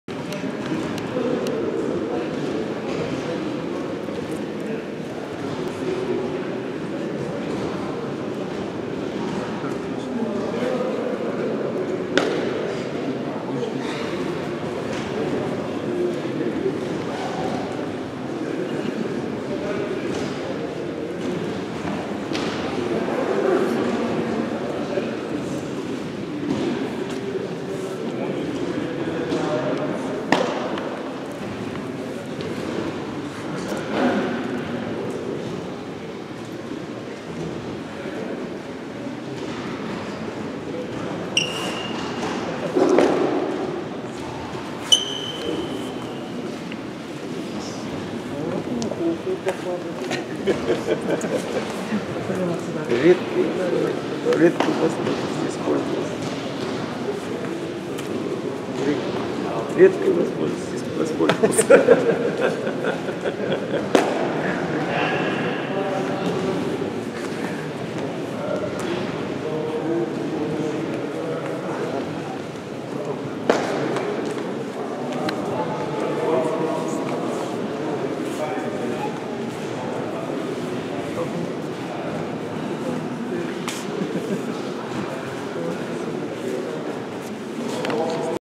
Такой же бородатый и великий. Как разминается Василий Левит
Серебряный призер Олимпиады в Рио-де-Жайнеро казахстанец Василий Левит готовится к выступлению на ЧМ в Гамбурге